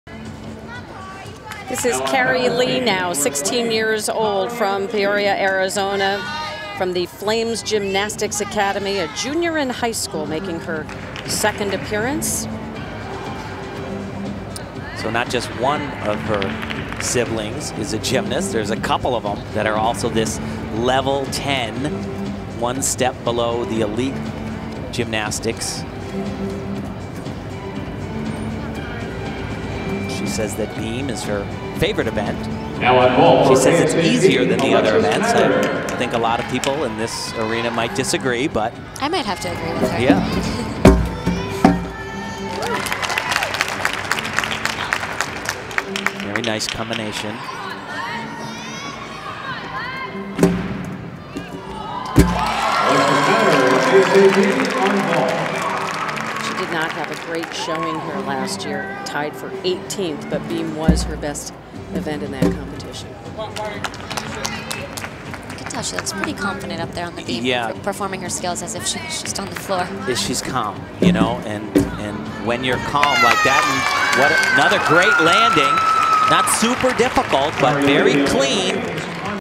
Kari Lee - Balance Beam - 2013 Nastia Liukin Cup
Score: 9.475
March 1, 2013 - DCU Center - Worcester, Mass.
Gymnastics